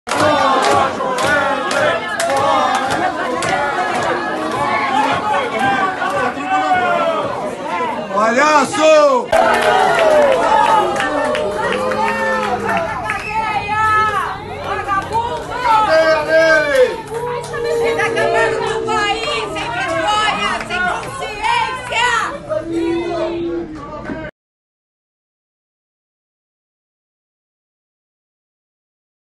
Wesley Batista é confundido com o irmão, Joesley, e sai de restaurante em SP com escolta
➡ Se você curte os conteúdos da Madeleine, apoie esse bom jornalismo. Assine